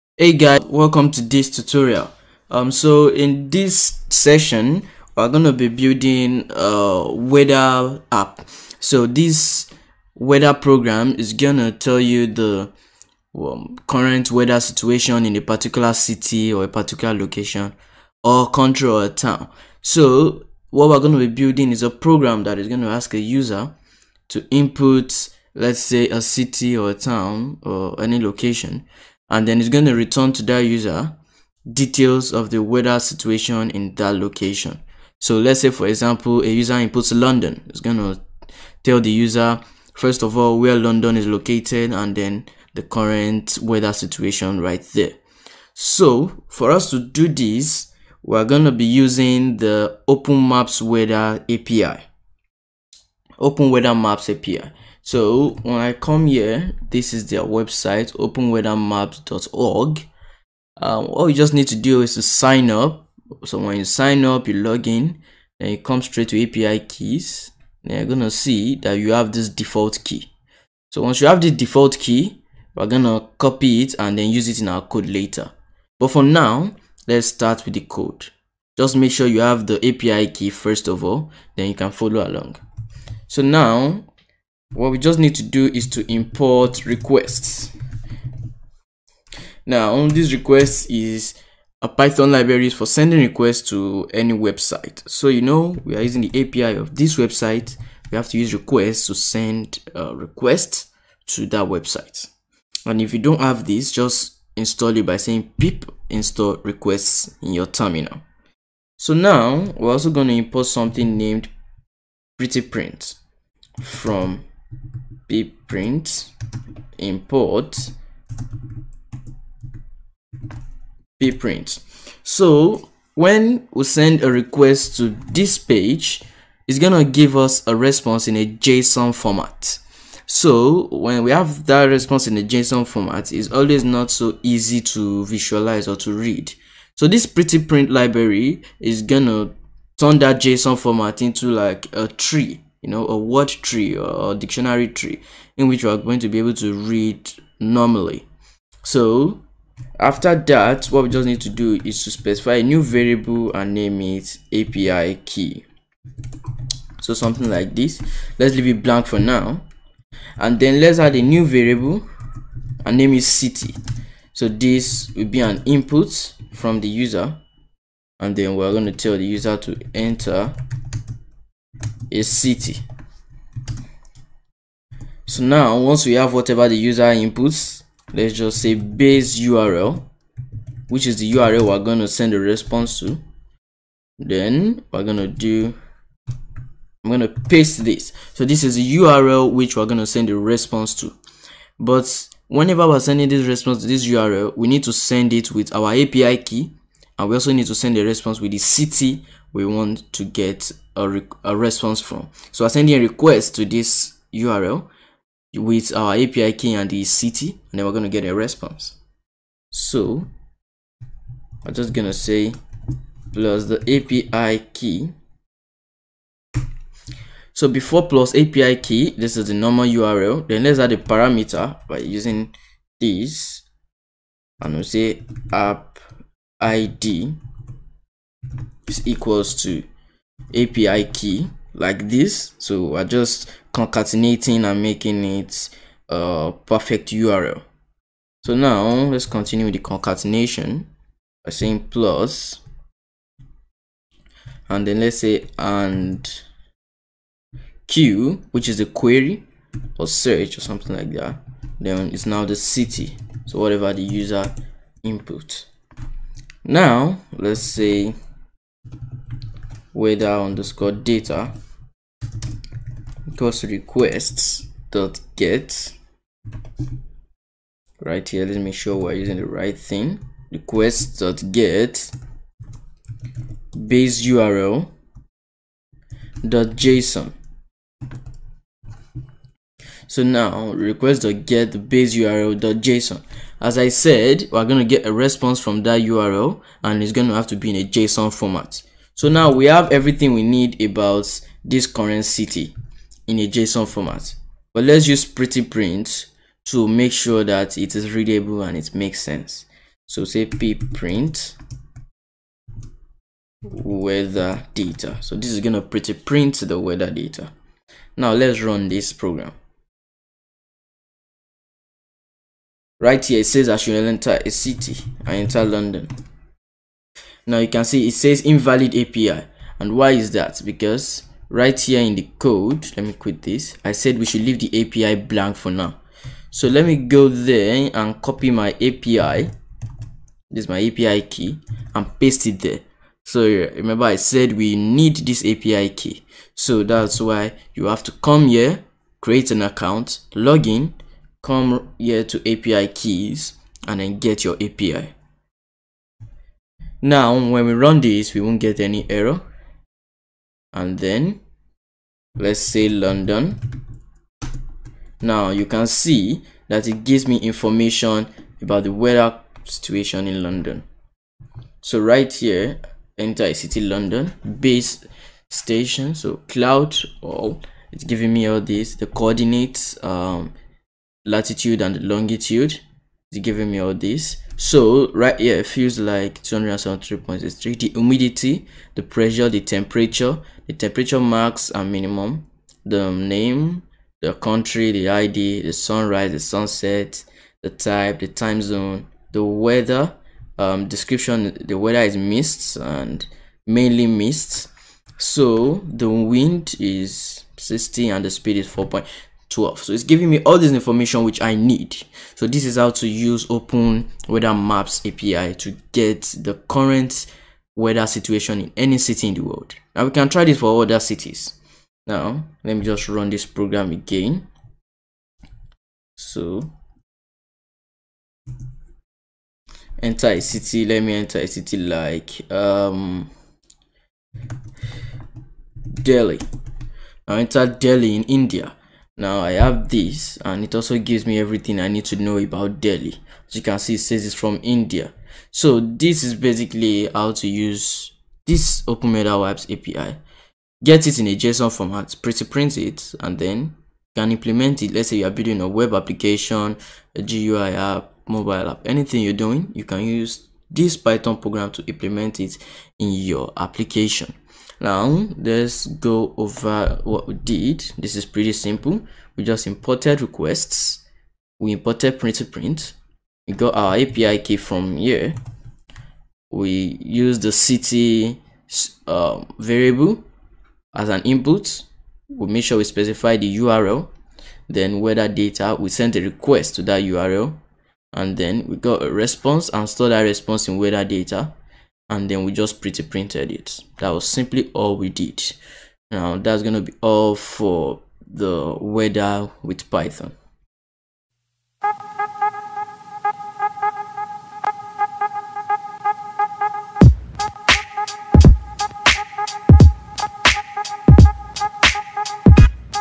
How To Build A Weather App With Python | Weather API
In this video, you will learn how to build a simple weather application using python and openweathermaps api.

___Youtube Gear___
Blue Yeti Microphone
Hp Windows 10
Wireless Mouse
Homfa Computer Desk
Mini Ring Lights
Canon EOS Camera